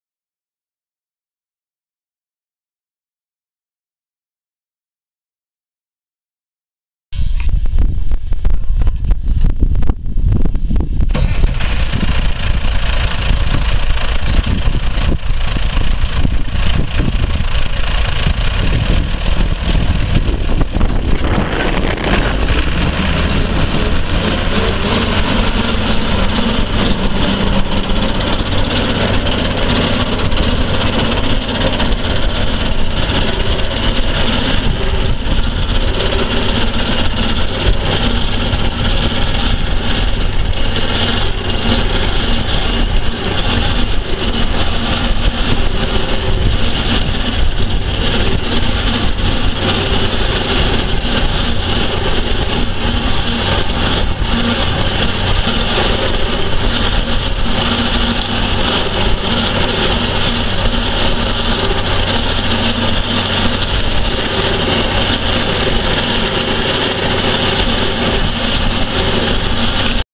Allis Chalmers 200 tractor for sale | sold at auction May 29, 2013
Allis Chalmers 200 tractor
2,581 hours on meter
Allis Chalmers six cylinder diesel engine
Manual transmission
8F-2R gears
Spring seat
Three point
Draw bar
Auxiliary outlet
540 PTO
Sidewinder 13&#039; rotary mower
205/75R14 tires
104" wheelbase
10.00-16 front tires
18.4-38 rear tires
Serial 2007021D

It was located in Balko, OK, and we sold it to the highest bidder for $4,070.